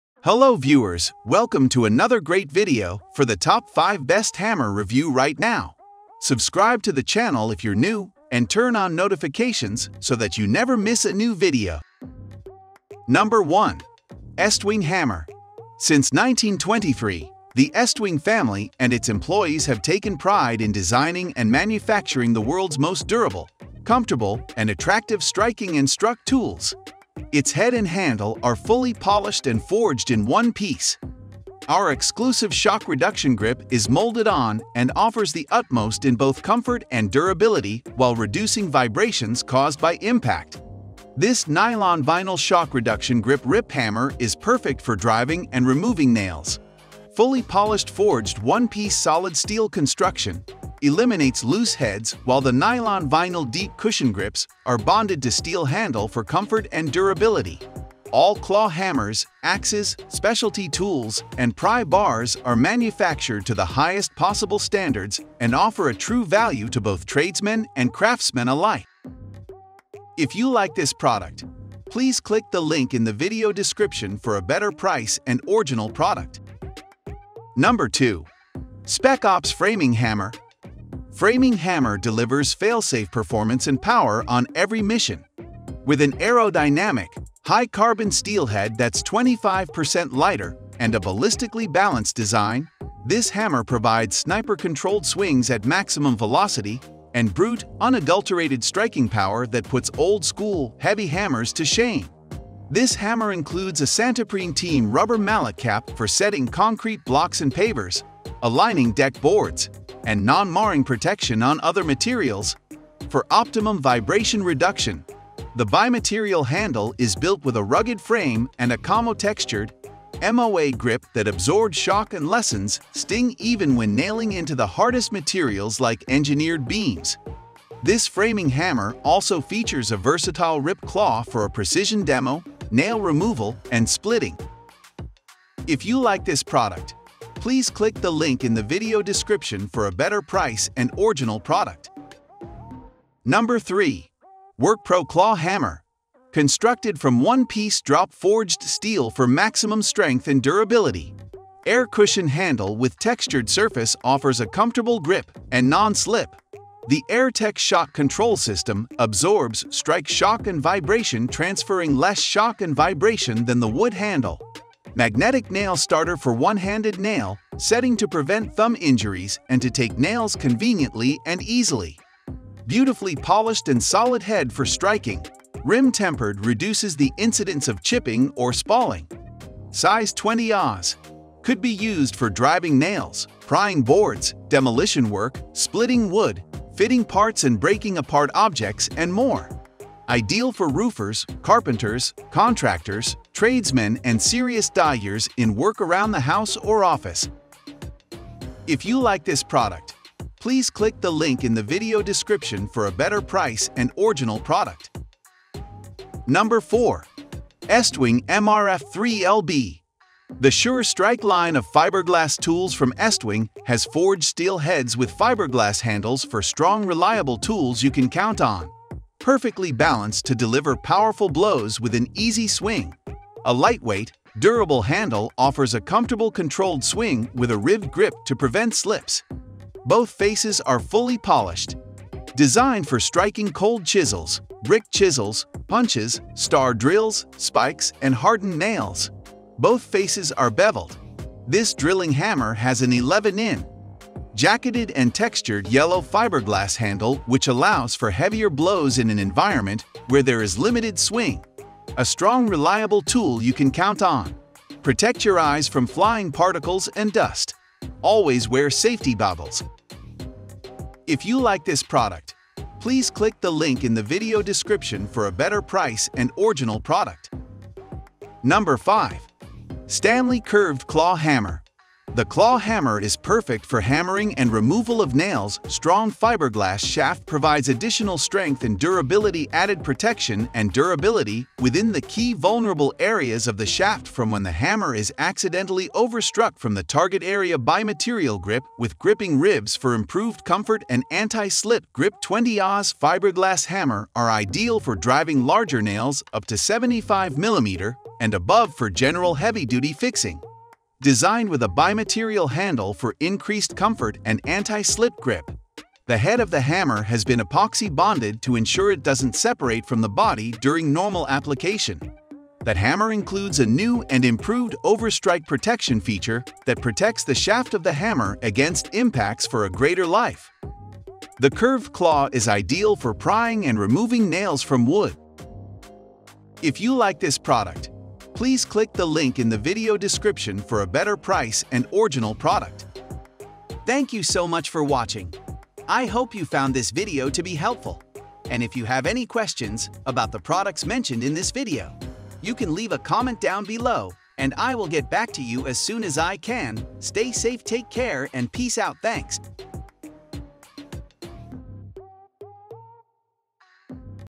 Top 5 Best Home Hammer Review In 2022
Top 5 Best Home Hammer Review In 2022 welcome to this video
1. ESTWING Hammer

2. Spec Ops Framing Hammer

3. WORKPRO Claw Hammer

4. Estwing MRF3LB

5. Stanley  Curved Claw Hammer